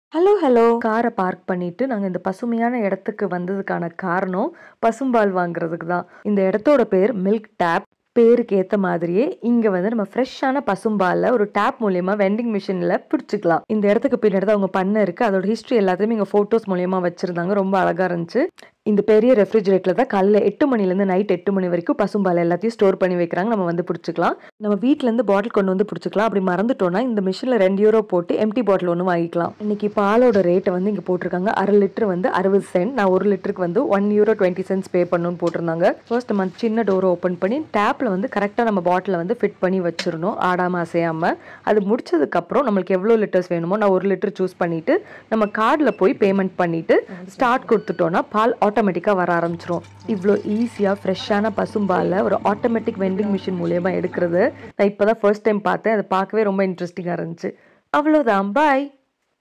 Fresh Milk from Automatic Vending Machine in Netherlands 🐮🥛- Melktap
Melktap - where you get fresh cow milk in the Netherlands.